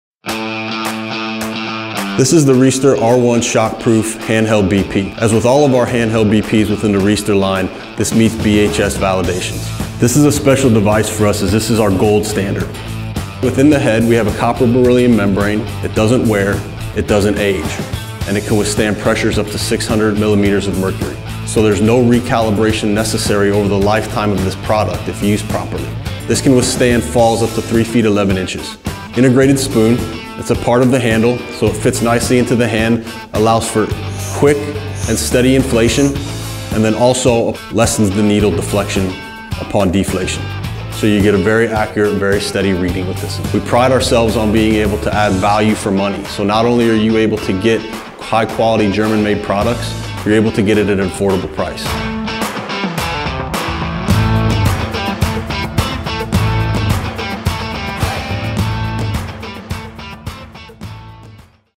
R1 Shock Proof Aneroid Sphygmomanometer
The R1 Sphygmomanometer Set is designed for ultimate accuracy and durability with innovative shock-proof technology.

Rehabmart was founded by two occupational therapists with a passion for helping people find answers to common healthcare struggles.